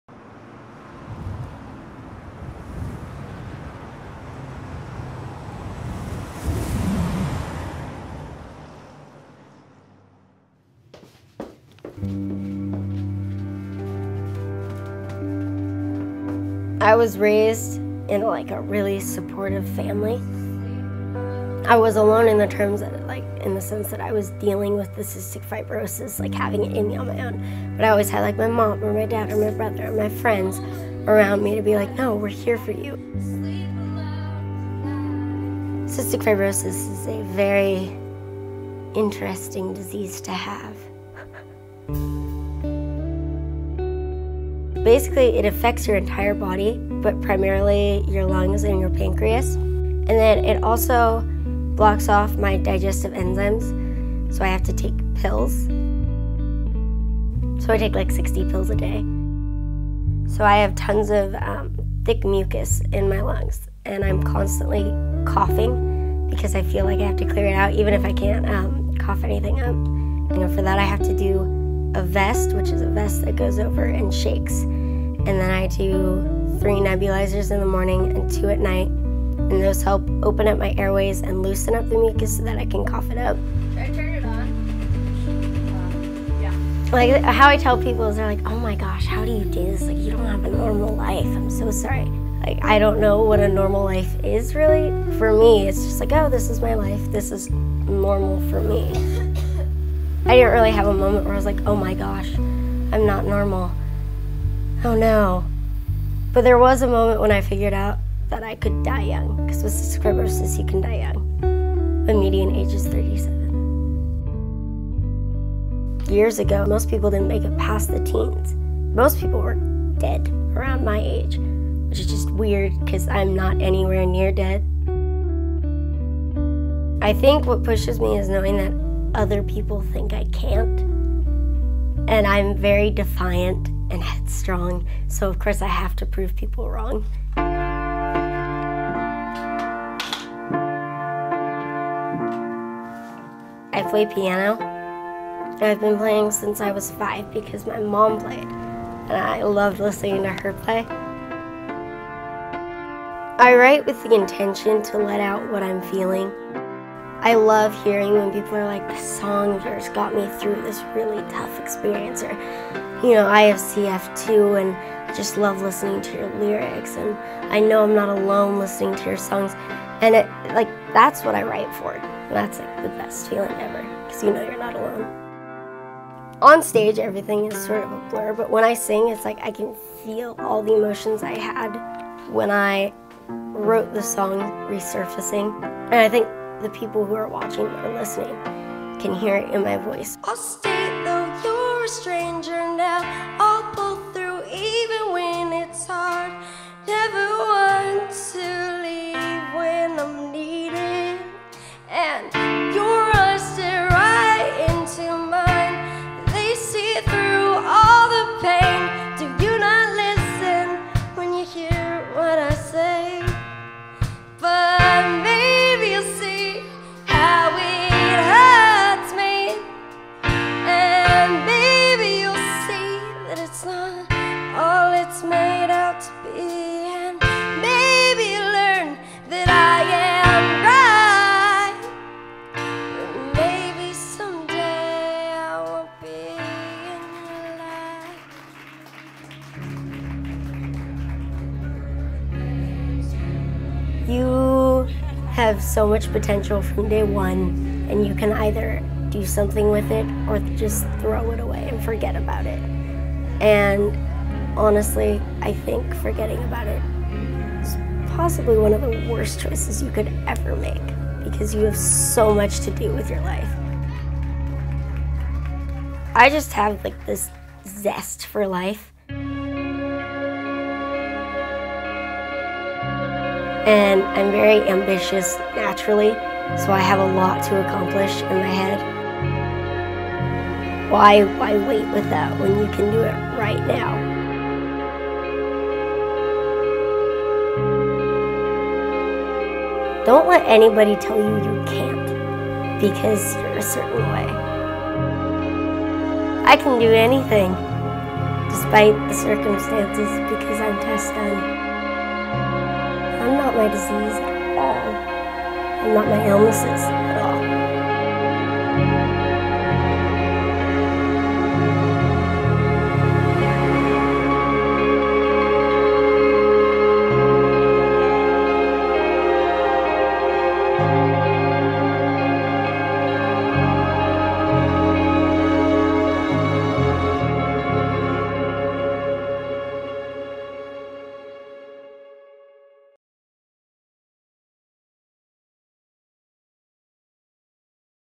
Pass the Bucket with Tess Dunn
Tess Dunn is a 17-year old singer-songwriter phenom who hails from Santa Cruz, California. Tess' raspy vocals, mature piano compositions and alternative sound transcend her years. She started playing the piano at age 5, began writing songs at age 11 and first took to the stage at age 12. Diagnosed with cystic fibrosis (CF), an incurable life-threatening disease which causes progressive lung damage, as well as epilepsy and diabetes, Tess faces issues of mortality that infuse her music and lyrics with radiant richness and haunting depth.

Tess has performed throughout California, including at the Vans Warped Tour at Shoreline Amphitheater in Mountain View in 2011 and 2010, and at the San Francisco Warped Tour in 2009. She has had successful solo shows in Santa Cruz County, and has opened for artists including Karla Bonoff, Nina Storey, Ari Shine, Adrienne Pierce, and Iris DeMent.

Tess says living with CF is "...almost like having a clock ticking over your head, but it has a way of putting things in perspective." She "finds a certain beauty in the randomness of life," and her music explores issues of love, trust and relationships. An affecting advocate for anyone battling the challenges of chronic disease, Tess encourages others due to to her take no prisoners zest for life.